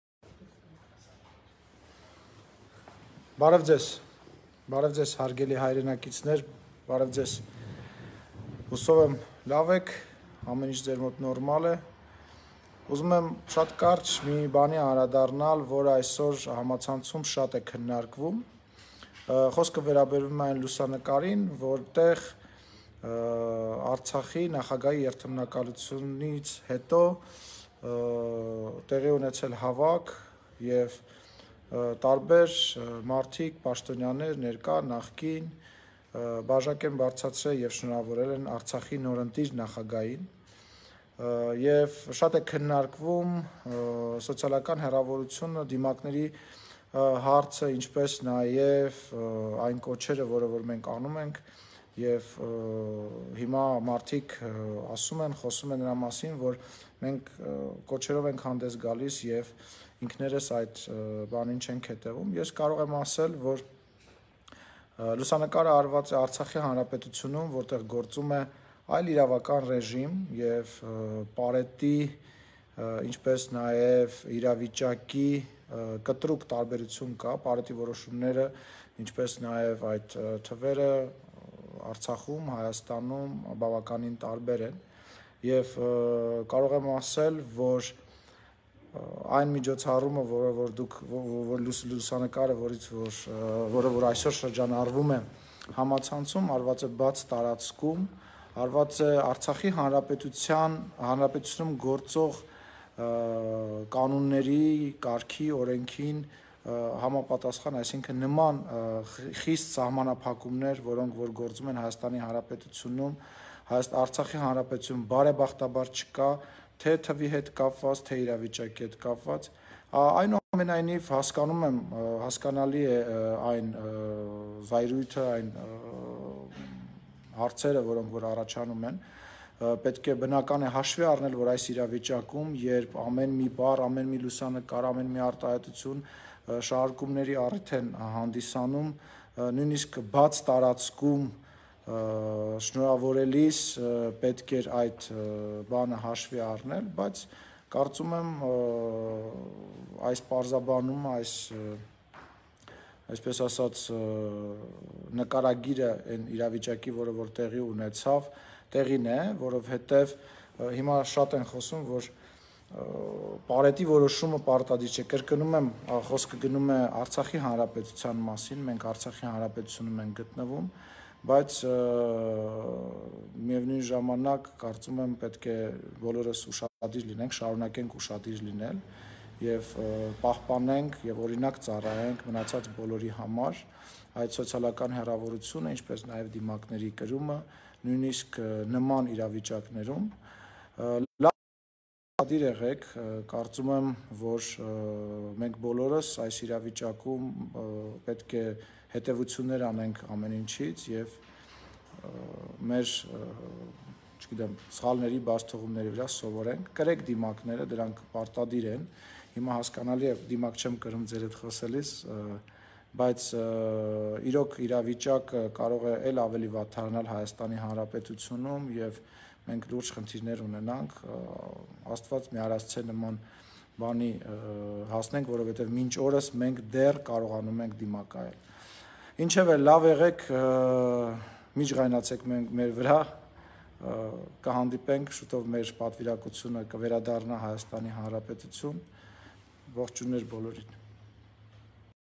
Ալեն Սիմոնյանը՝ քննադատված լուսանկարի մասին
ԱԺ փոխնախագահ Ալեն Սիմոնյանն իր ֆեյսբուքյան էջի ուղիղ եթերում անդրադարձել է երեկ սոցցանցերում քննադատության արժանացած հայտին լուսանկարին։
Լուսանկարում երևում է, թե ինչպես են ՀՀ իշխանության ներկայացուցիչներն առանց դիմակի և սոցիալական հեռավորություն պահելով շնորհավորում Արցախի նորընտիր նախագահ Արայիկ Հարությունյանին։